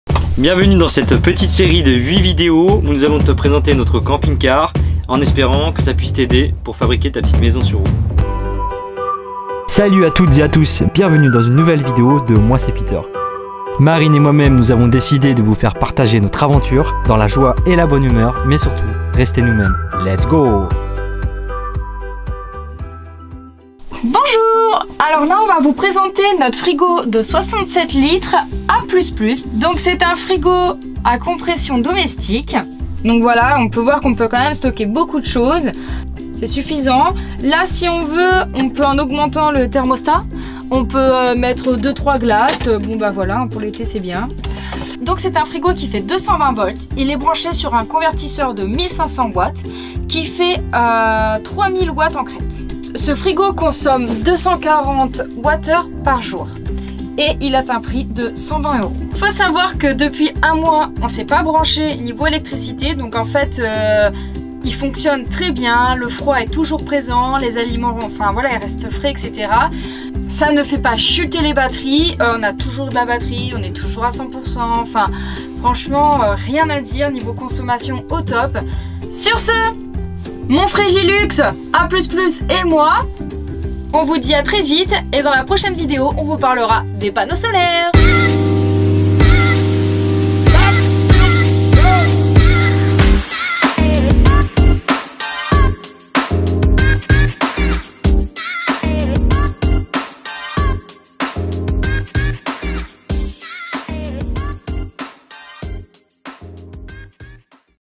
Frigo 220v dans camping-car Ep1/8
Dans cette série de petites vidéos nous vous présentons notre camping-car en détail. Nous avons fait 3 mois de travaux , et nous avons aménagé le camping-car à notre façon. En espérant que ça puisse vous aider !!!
Si tu as besoin de plus d'info , contacte nous sur notre page facebook " Les aventuriers du globe "
Pour nous soutenir dans notre aventure et notre projet voici notre page TIPEEE :